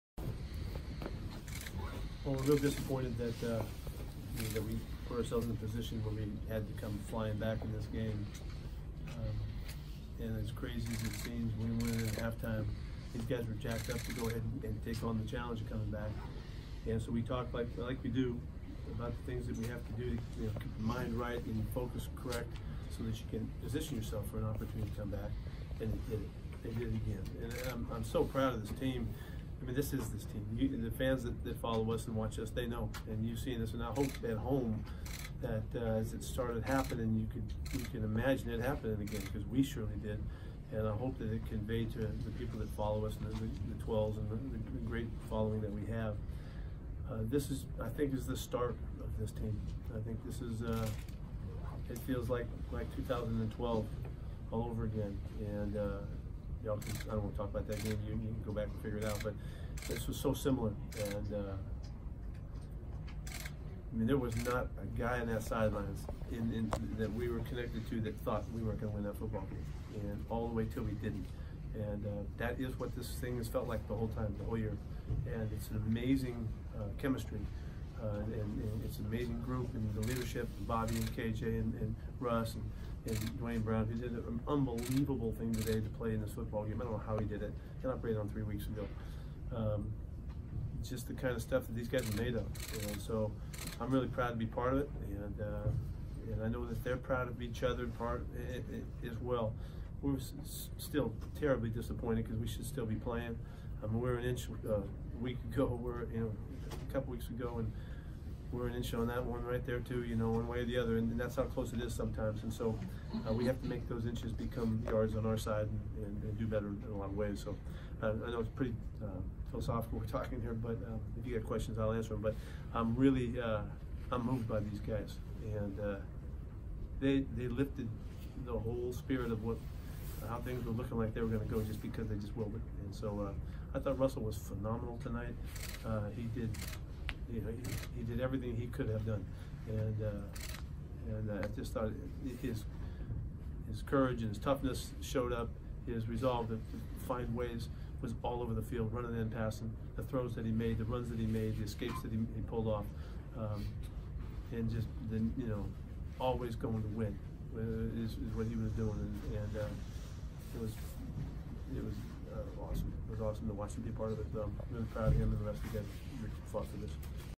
Pete Carroll’s postgame message after Seahawks’ rally falls short at Green Bay: pride
Coach Pete Carroll’s postgame message after Seahawks’ rally falls short at Green Bay: pride.

Video by Gregg Bell/Tacoma News Tribune